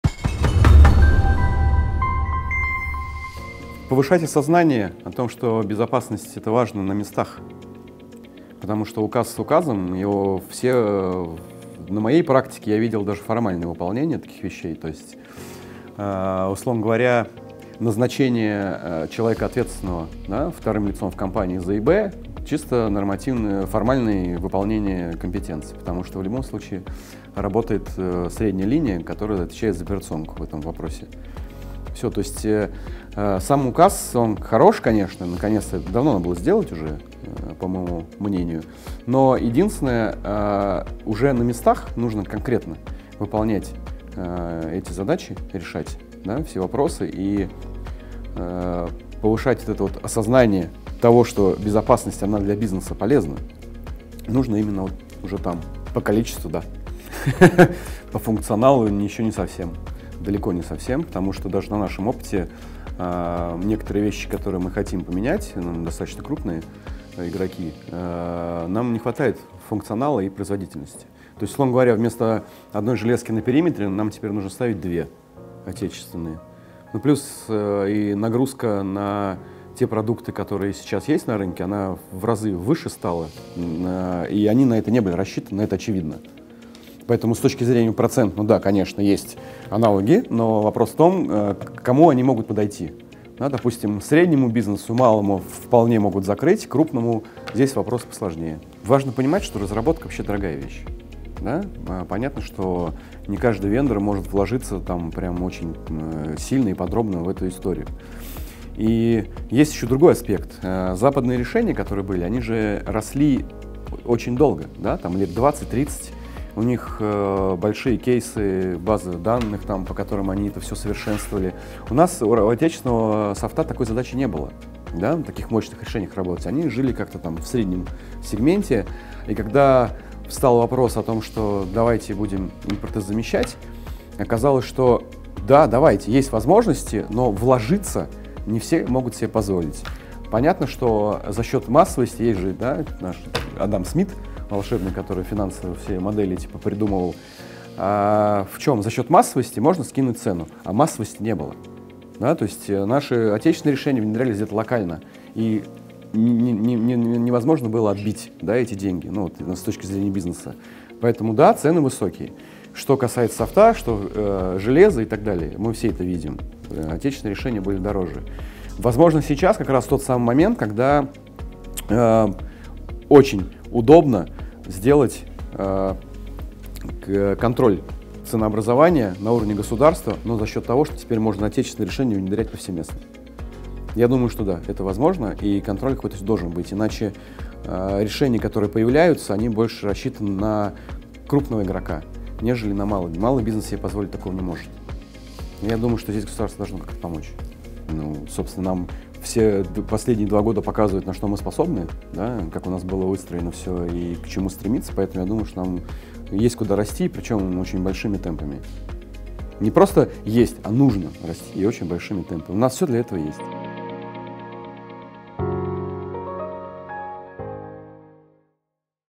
Как проходит процесс импортозамещения в российском кибербезе — Артём Калашников
Артём Калашников, директор проектов в «Газпромбанке», отмечает важность безопасности «на местах» — в пику формальному исполнению положений президентского указа № 250 и критически подходит к анализу процесса импортозамещения в российском кибербезе.

Рассказывает эксперт и о роли государства в вопросе регулирования цен на отечественные ИБ-решения и технологической «юности» продуктов российских вендоров — их прошлой жизни в «среднем сегменте» и чисто локальном, не-массовом применении.